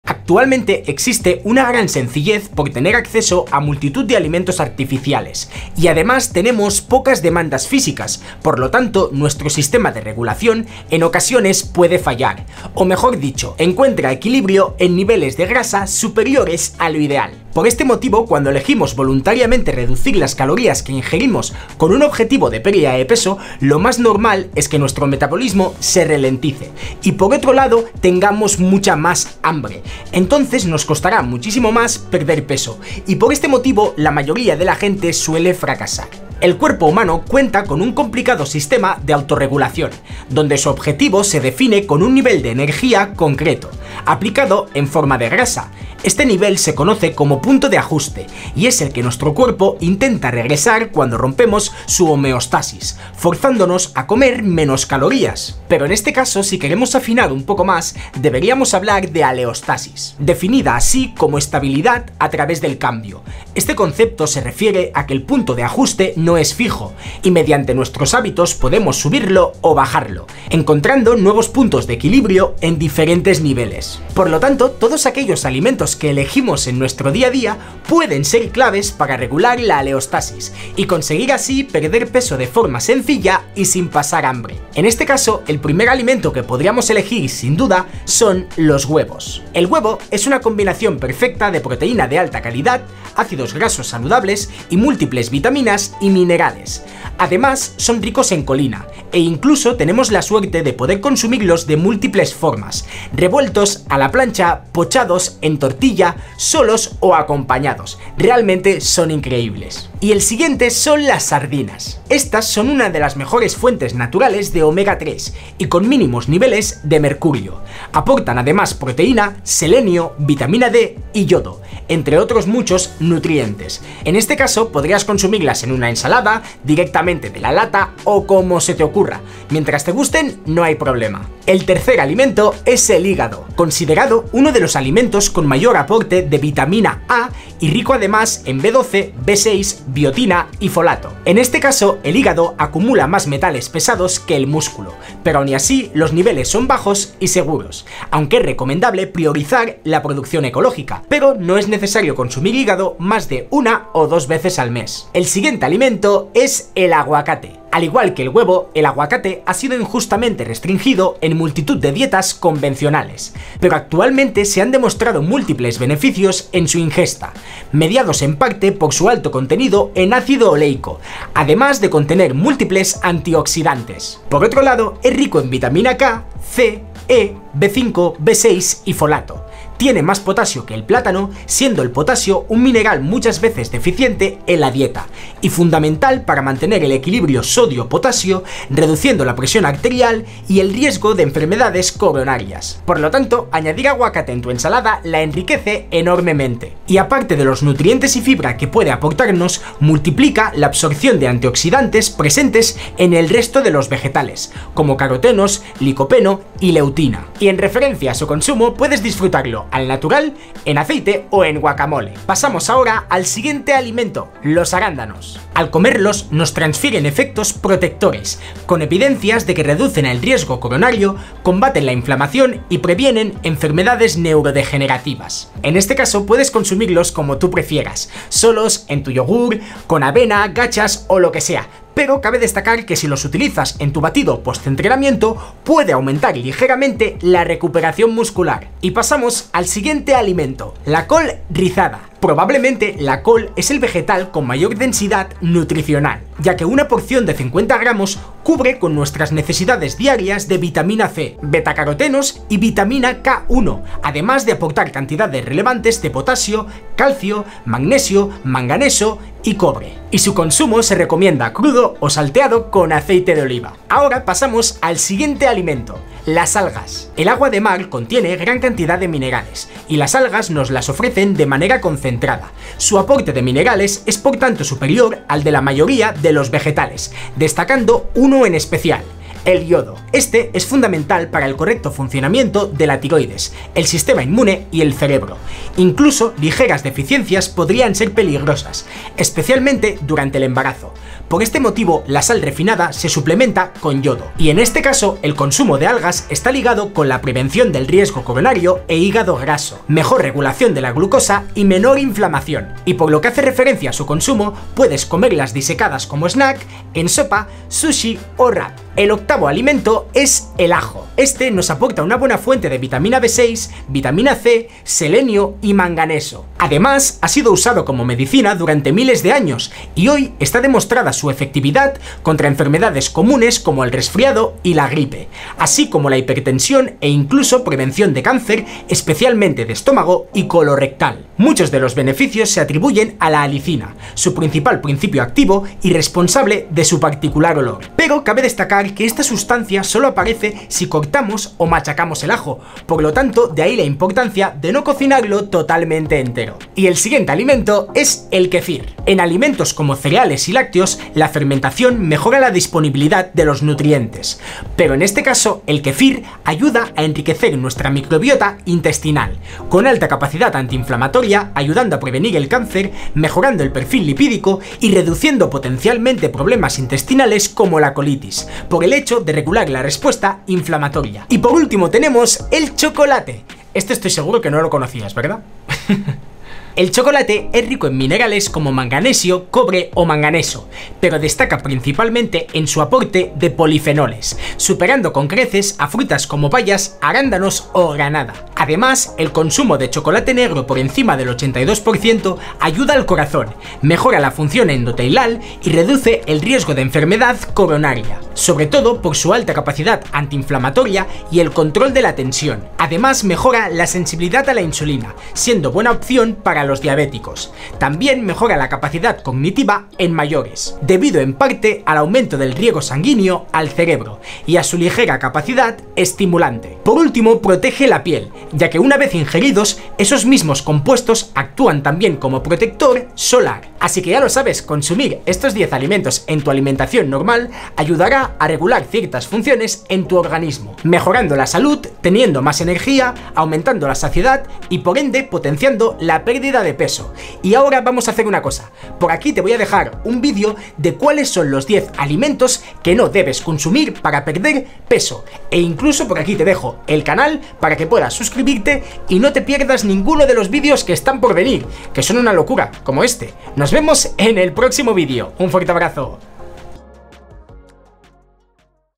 10 ALIMENTOS QUE DEBES COMER PARA PERDER GRASA (con evidencia científica)
En este vídeo aprenderás cuales son los 10 alimentos que debes comer si tu objetivo es perder peso. Añádelos en tu alimentación y me cuentas los resultados.

LA MÚSICA QUE USO EN MIS VÍDEOS (SIN COPYRIGHT)

➡ Camara SONY APHA 6600
➡ Camara de ACCIÓN (GOPRO 8)